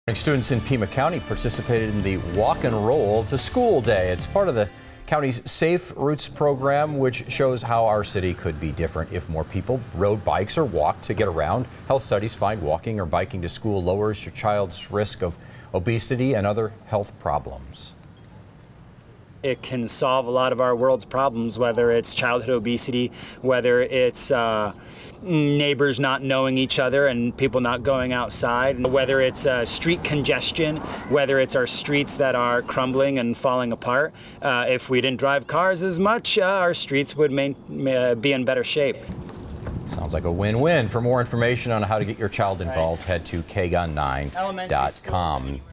Walk 'N Roll encouraging students to walk and bike to school
On Friday, more than 50 different schools across Tucson participated in a Walk 'n Roll. Pima County encouraged students and their families to ditch the car and get to school on a bike or by walking instead.